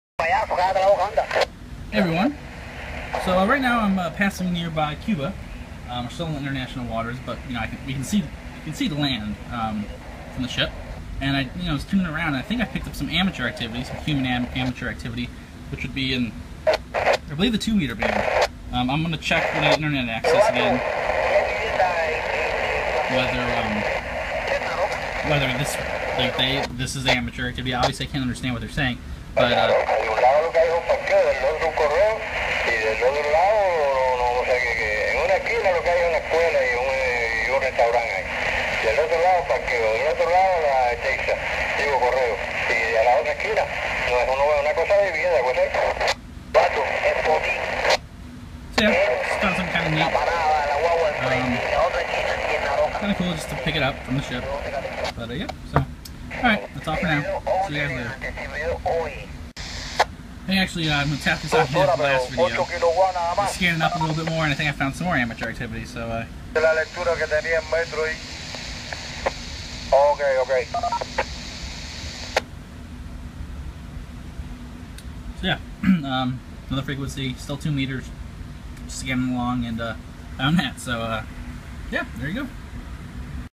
Cuban Amateur Radio Activity while on Disney Cruise
While passing Cuba on my cruise (about 10 miles from shore), I scanned around and found some Cuban amateur radio activity on 2 meters.